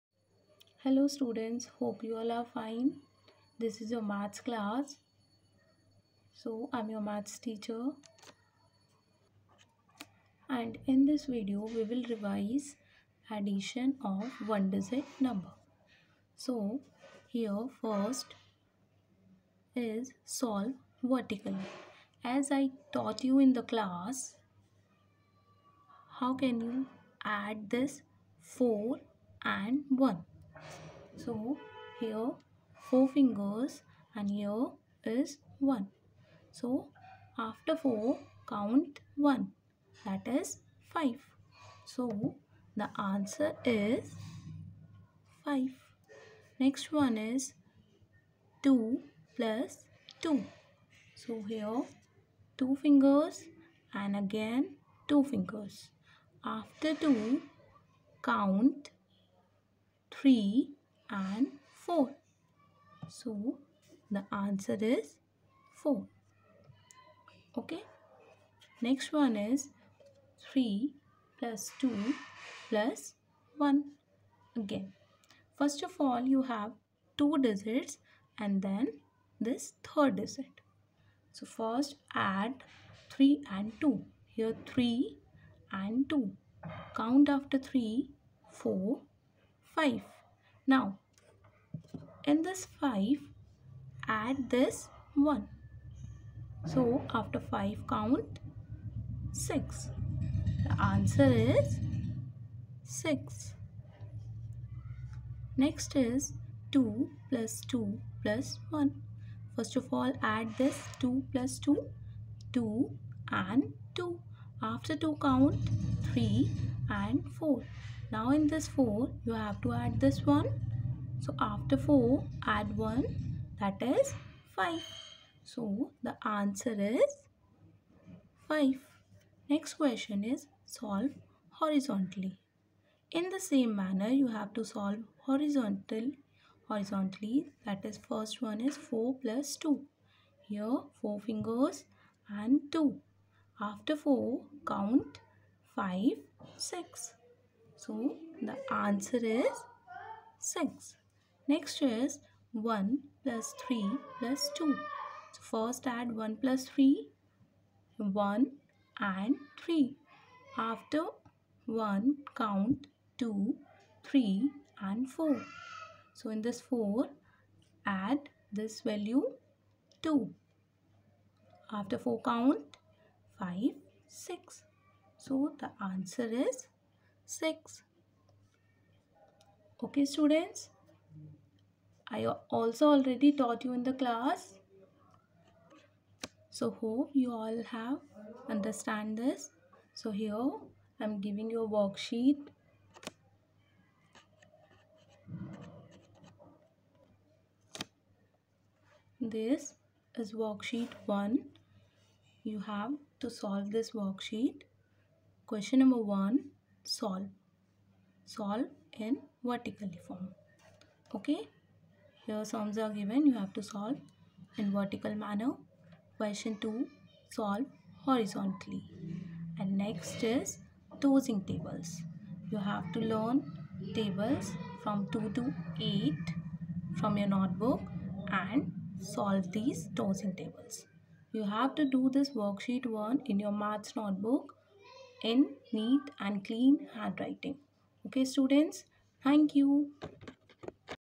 Class 1Maths
(8/7/23) Revision of addition of one - digit numbers .